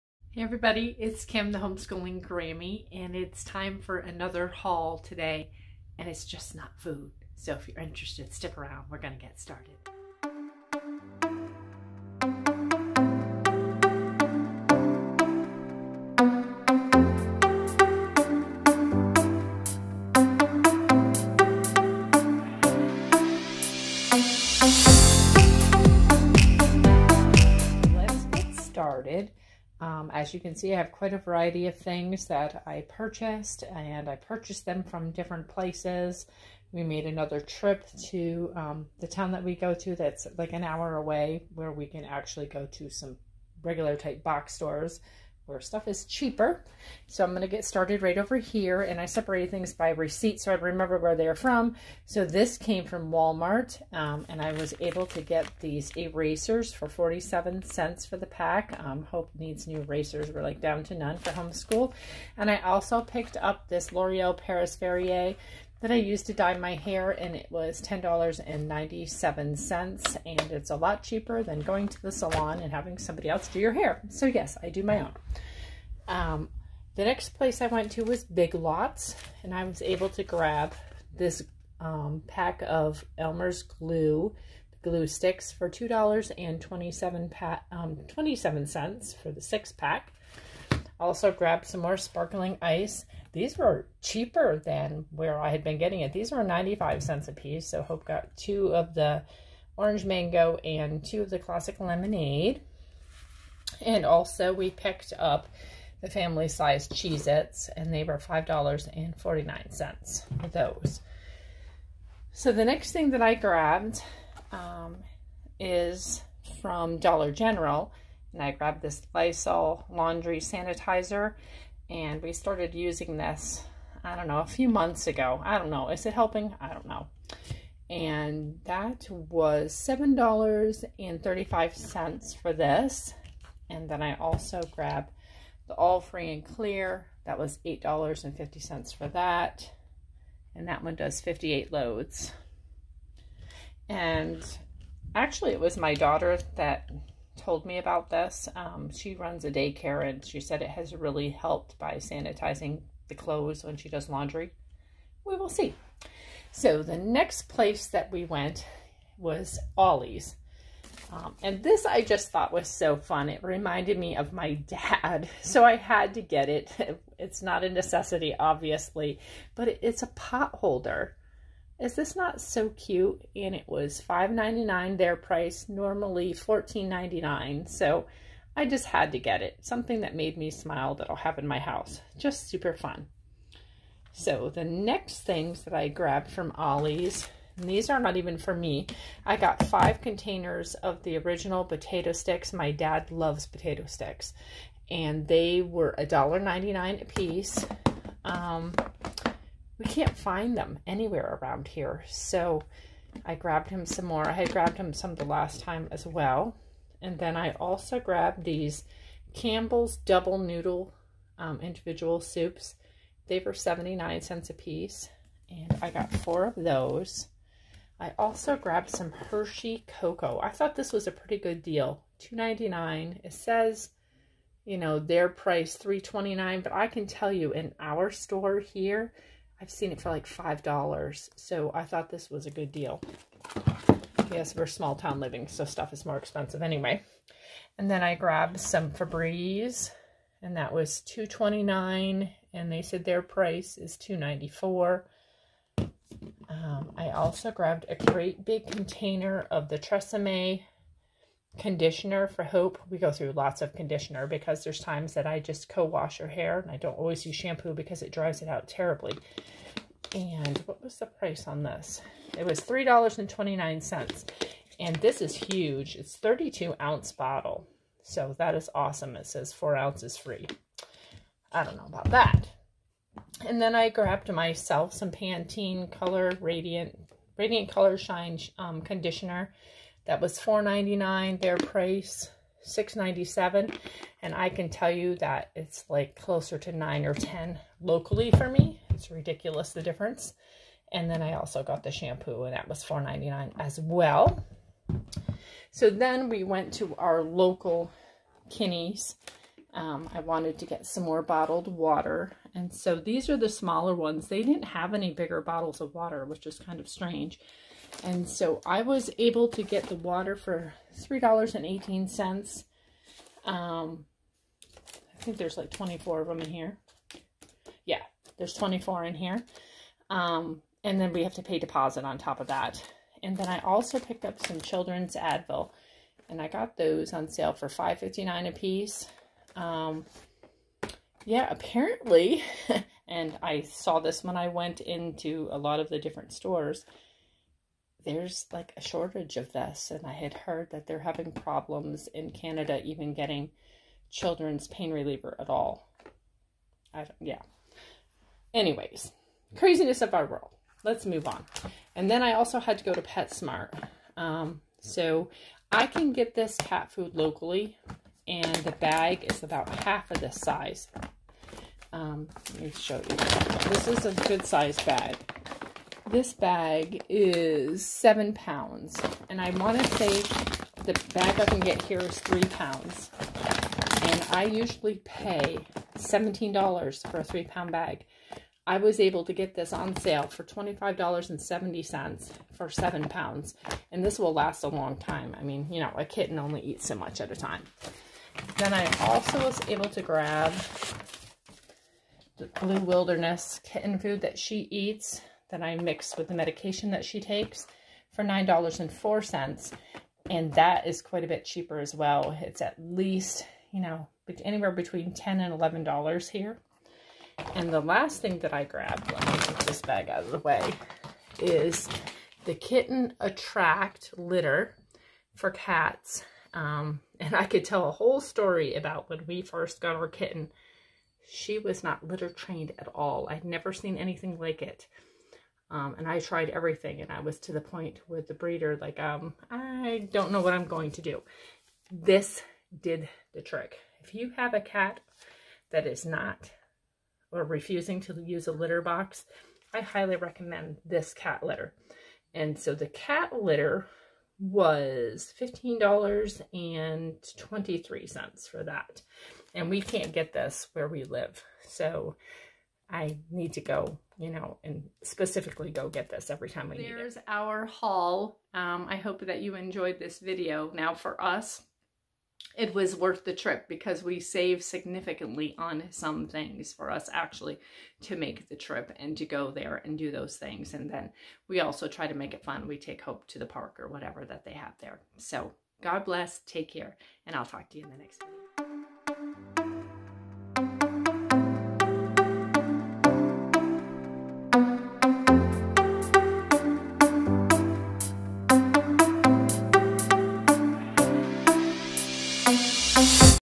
Walmart / Ollie's / Big Lots/ Haul
It's a haul day!! Wlamart, Olllies and Big Lots! Come see all my goodies. God Bless